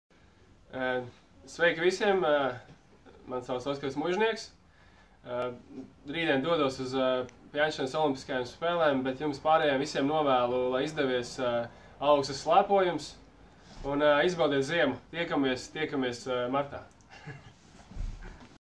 Oskars Muižnieks pirms Olimpiādes aicina uz dzimto Alūksni
Olimpietis Oskars Muižnieks aicina uz savu dzimto pilsētu Alūksni izbaudīt ziemu un piedalīties Moller's Slēpošanas seriāla "Latvijas Ziema 2018" 2.posmā, tautas slēpojumā Alūksne, sporta bāzē Mežinieki! Izmēģini arī Tu Oskara slēpošanas trases! Piesakies